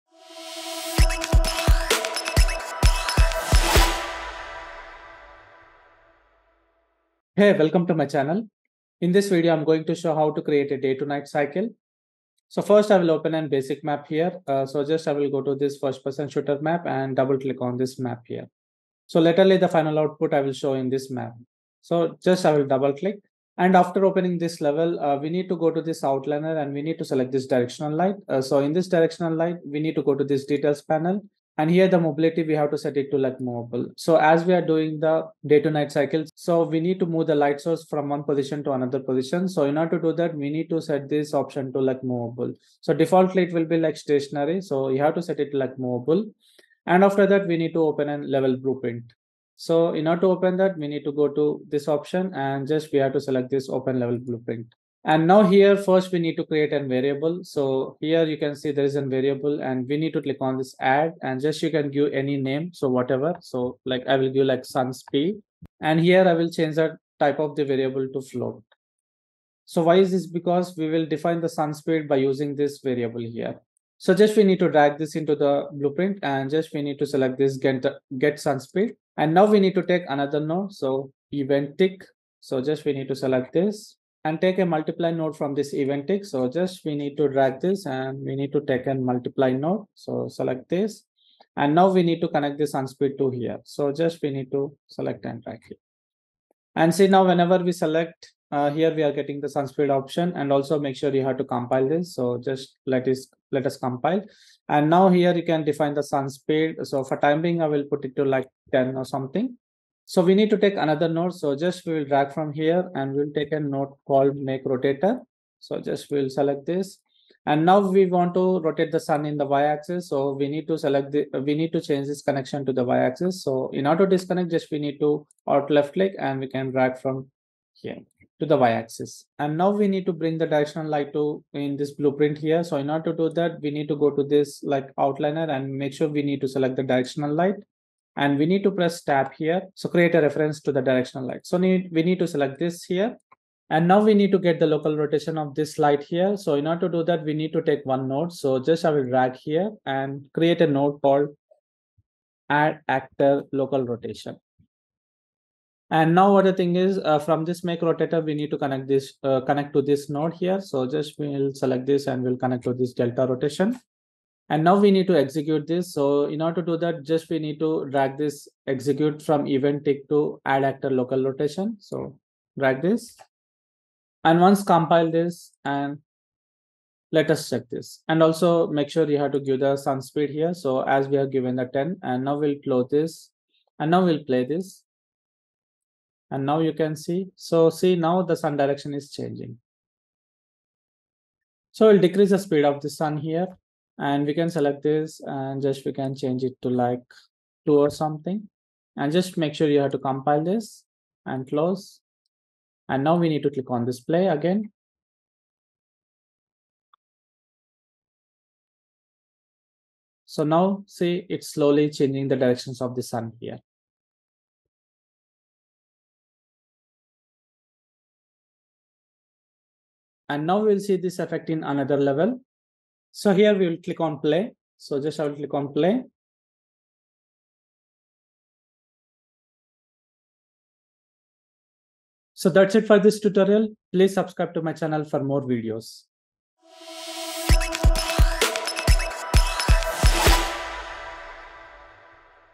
Unreal Engine 5: Master the Day to Night Cycle in Simple Steps
In this video, we're going to walk through the simple steps to creating a day to night cycle in Unreal Engine 5. This cycle will allow your scene to transition from daytime to nighttime without any user input, making it easy to add a realistic night time feel to your game or scene.